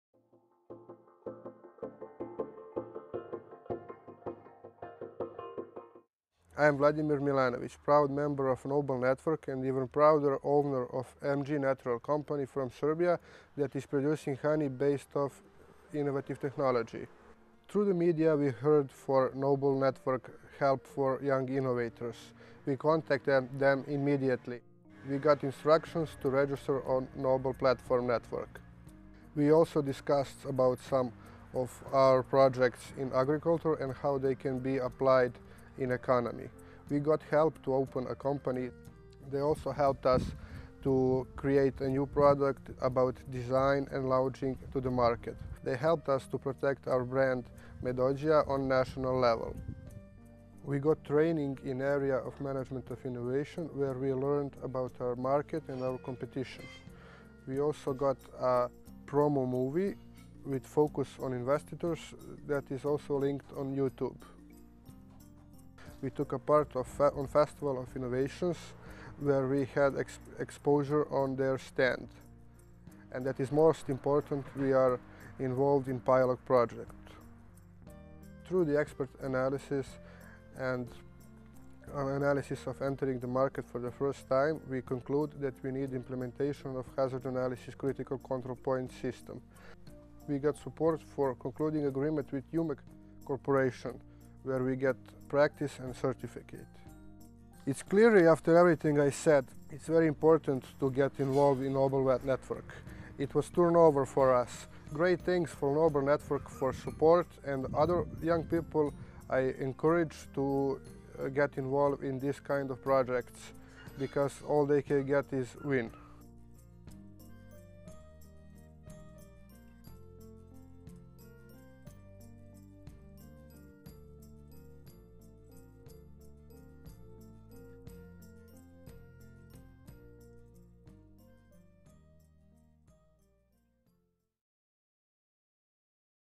Thank You NO-BLE Ideas Project
Movie for thanking NoBle Ideas Project, in association by chamber of commerce and industry Serbia.